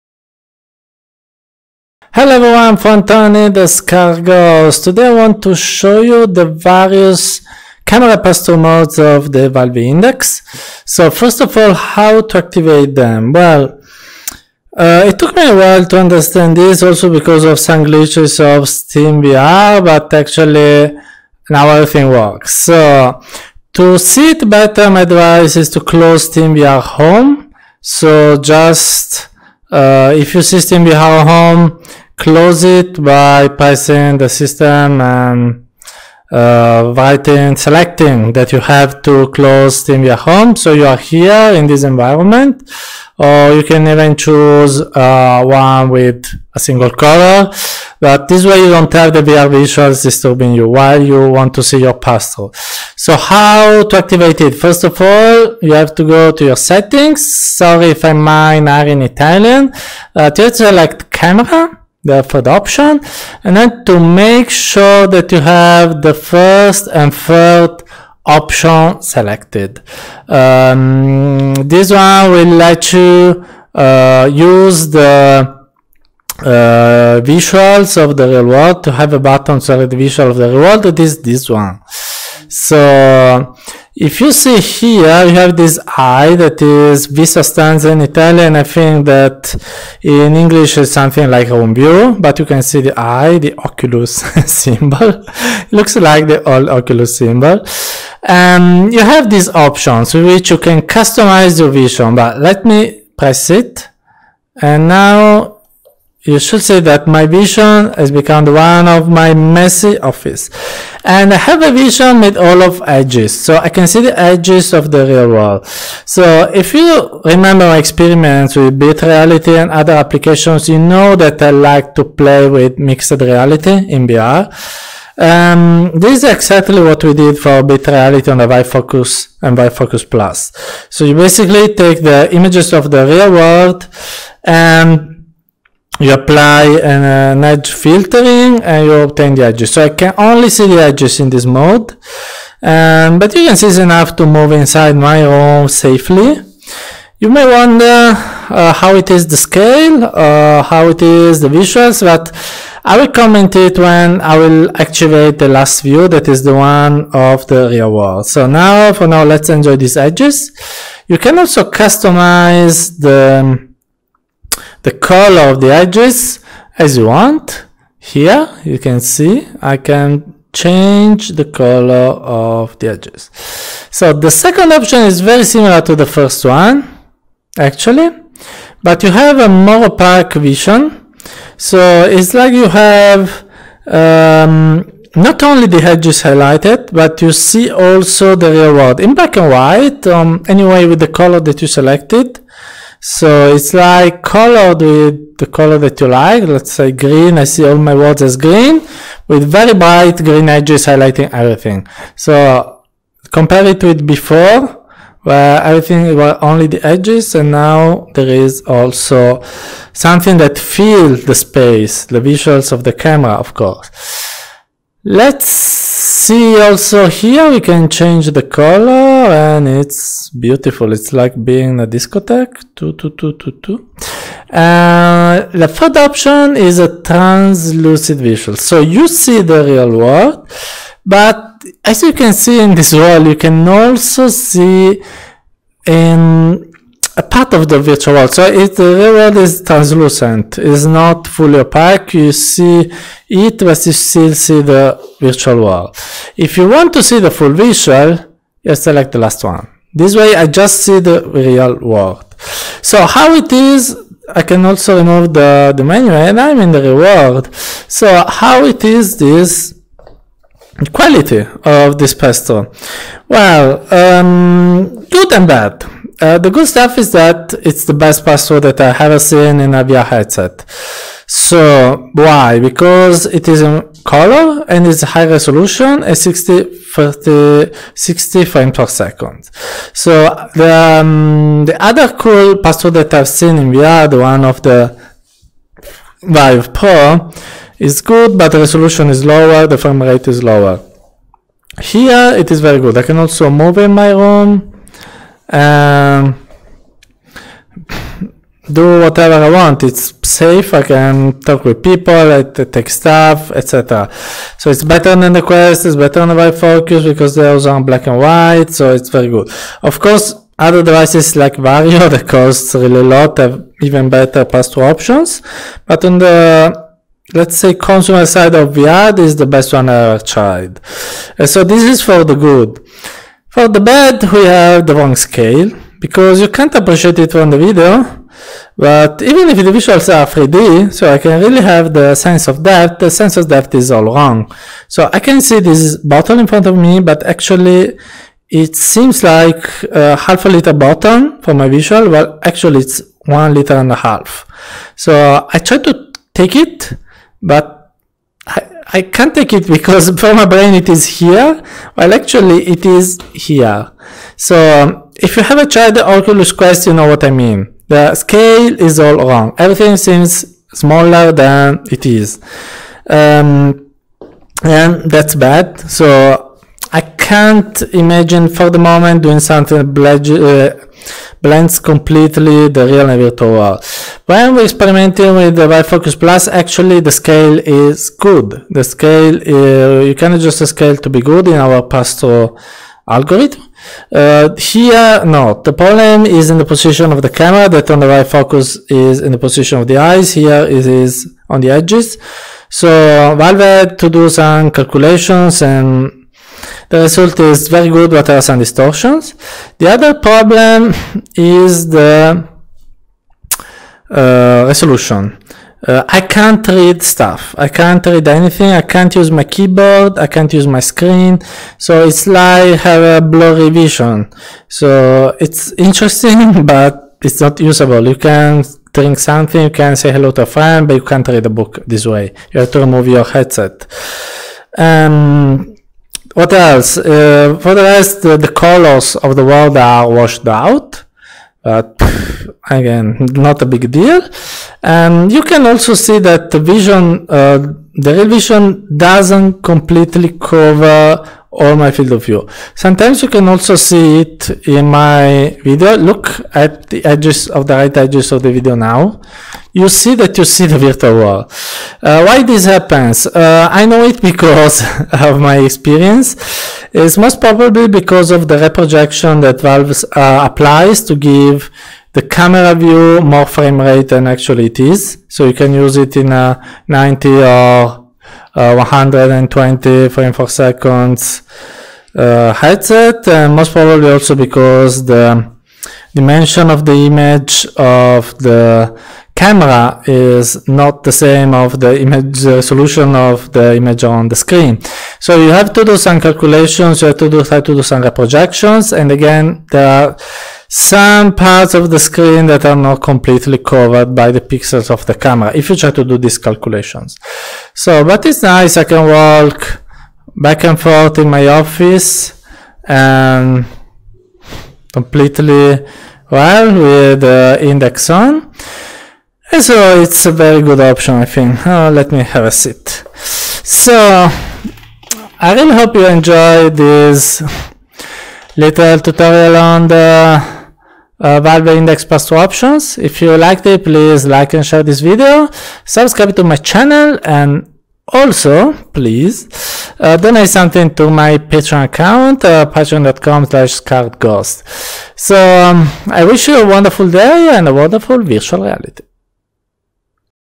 How to test the different passthrough modes on the Valve Index
This video will tell you everything you need to know on passthrough vision on the Valve Index:
- How to activate the cameras;
- What are the various mixed reality modes and what are their differences and their options;
- What are my impressions on this mixed reality vision